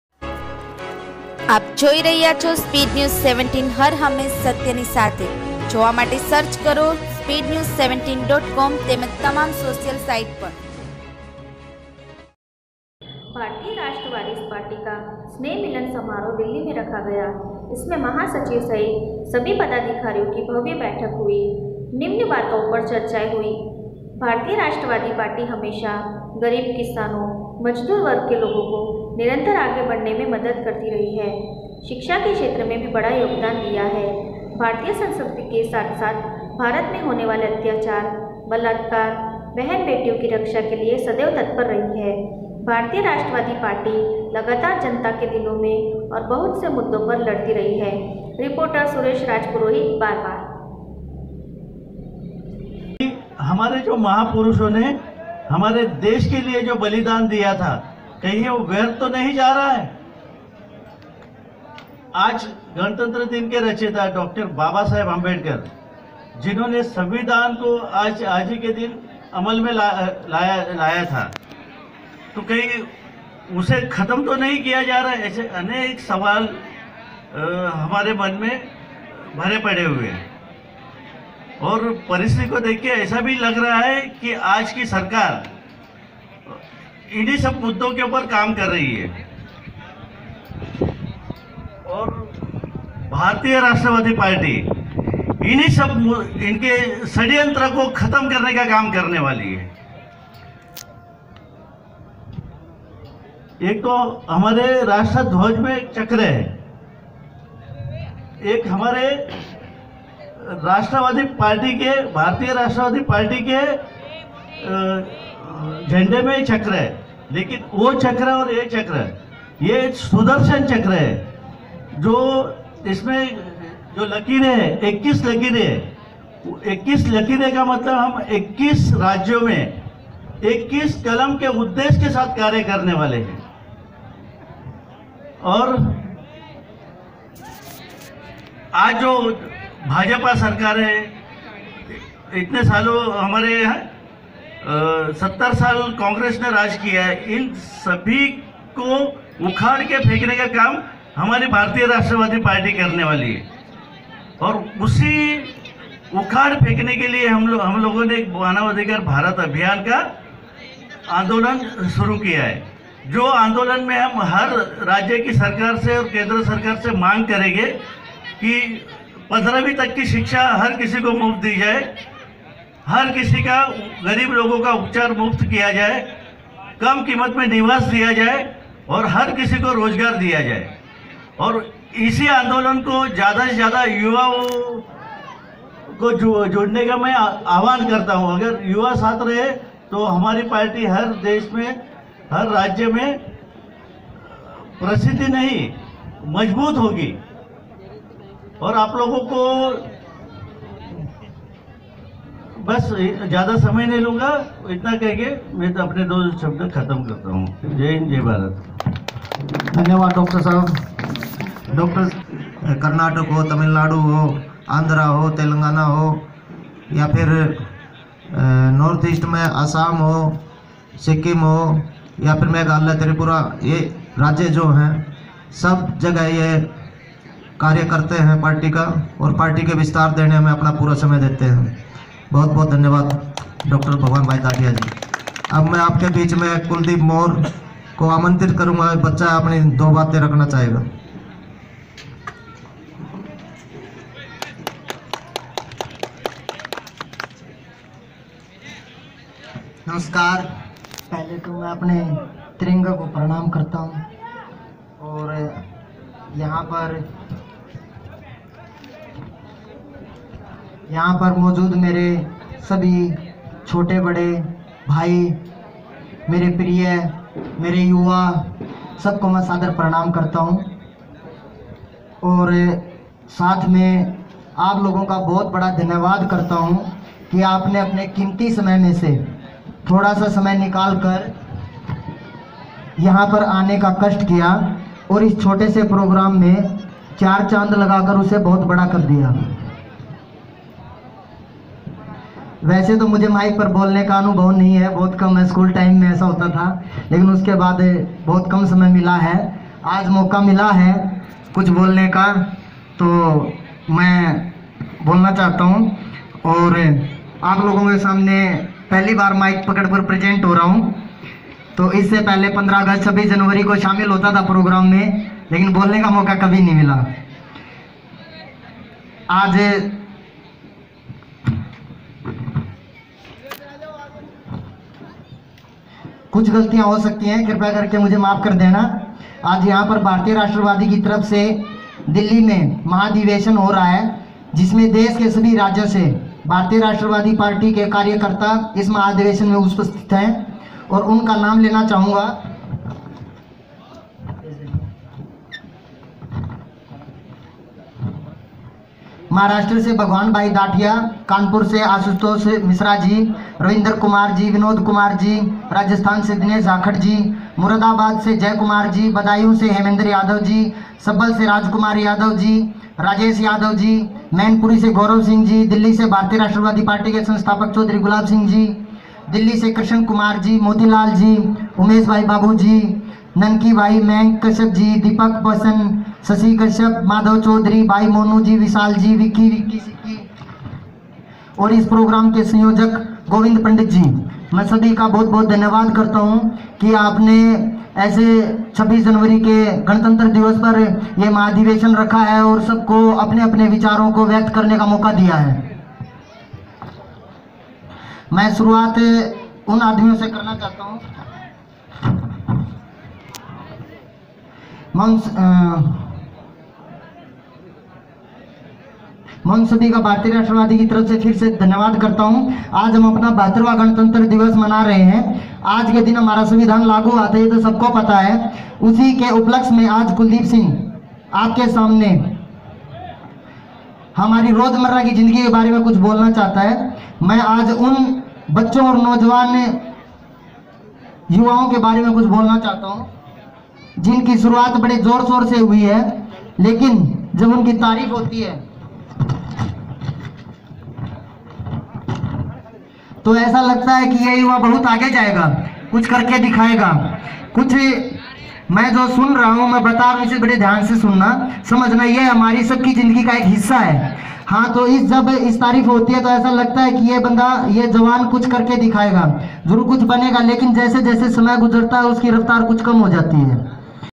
DELHI भारतिय राष्ट्र वादी पार्टी का सहने मिलन समारोह दिल्ली में रखा गया इसमें महासचिव सहित सभ
भारतिय राष्ट्र वादी पार्टी का सहने मिलन समारोह दिल्ली में रखा गया इसमें महासचिव सहित सभी पदाधिकारियों की भव्य बैठक हुई निम्न बातों पर सरचाए हूई भारतीय राष्ट्र वादी पार्टी हमेशा गरीबों किसानों मजदुर वग के लोगों को निरन्तर आगे बढ़ाने में मदद करती रही है शिक्षा के क्षेत्र में  भी बड़ा योगदान दिया है भारतीय संस्कृति के साथ साथ भारत में होने वाले अतियाचार बलात्कार बहिन बेटियों की रक्षा के लिए सदैव तत्पर रही है भारतीय राष्ट्र वादी पार्टी लगातार  जनता के दिलों में और बहुत से मूदो पर लडती रही है           रिपोर्टर सुरेश राजपुरोहित बारवा